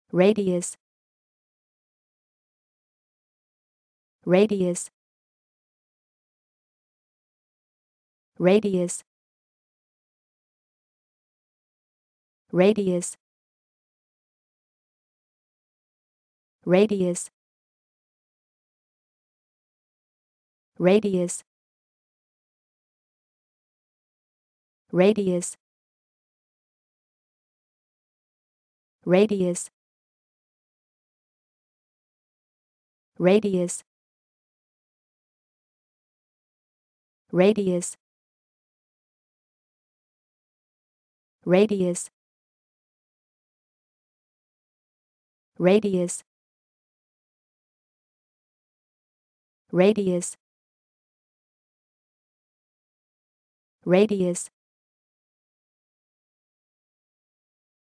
Como dizer 'raio' em ingles?
Como dizer raio em Ingles? Como se pronuncia radius? How to pronounce radius?